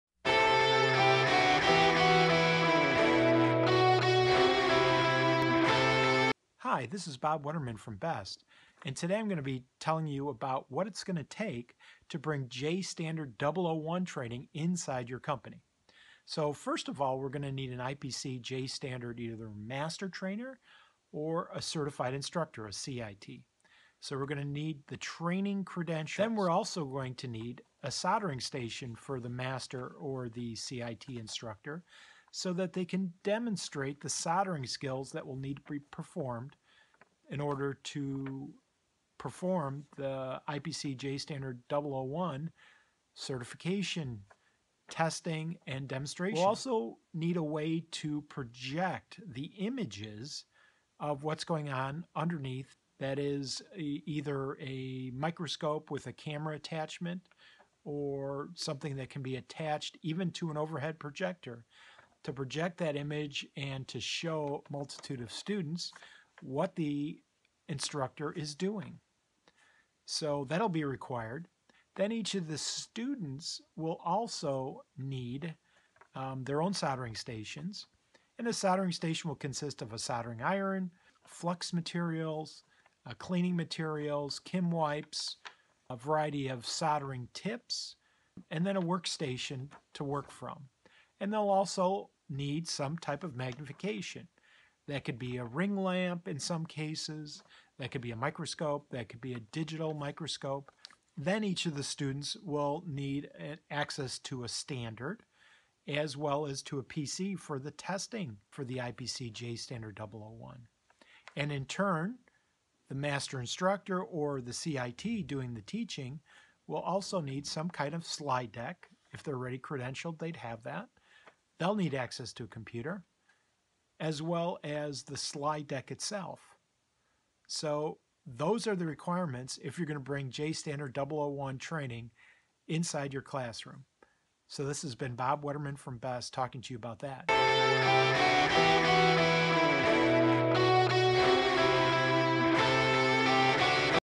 What it takes to start J-STD-001 Training
This video describes what you will need to bring IPC J-STD-001 training inside your facility.  This lost includes: J-STD-001 standard, soldering stations, J-STD-001 slide deck, overhead projector and certified trainer. For greater detail please check out or look here for J-STD-001 certification training